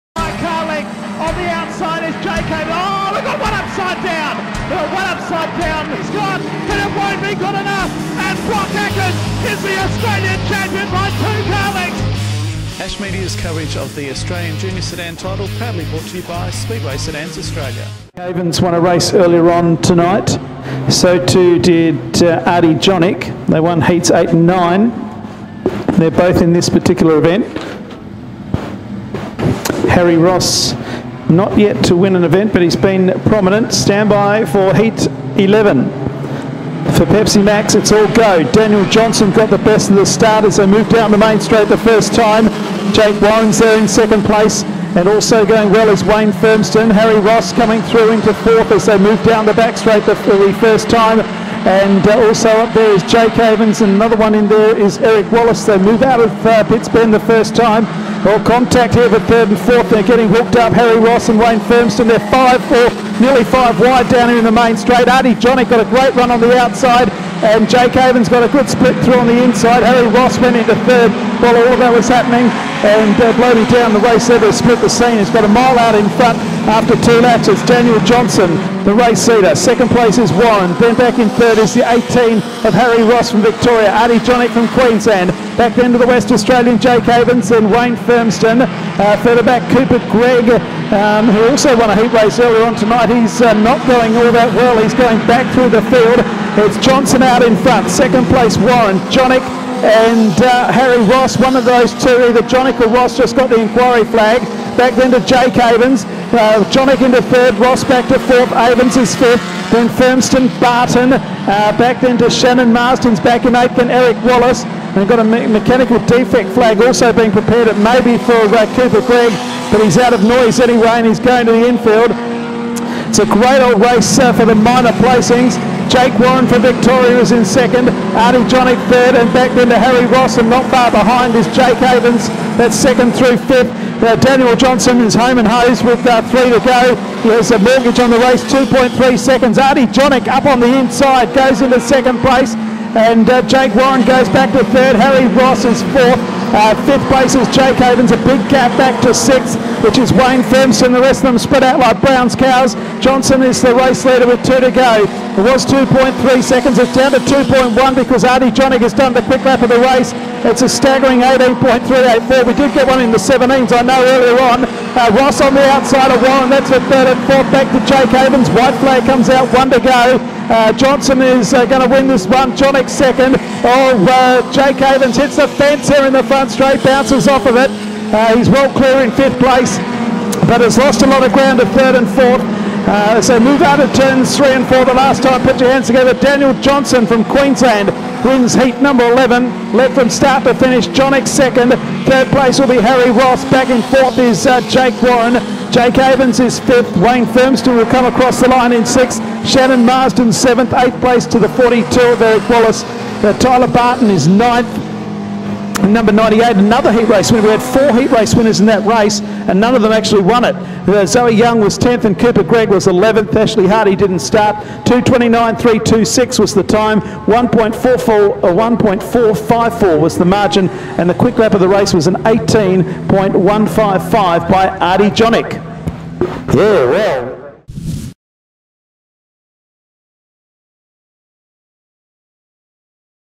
Junior Sedans - Australian Title - Heat 11 - Carrick Speedway - 08.01.16
This is Heat 11 of the Pepsi Max Australian Junior Sedan Title held at Cranes Combined Carrick Speedway on the 08.01.16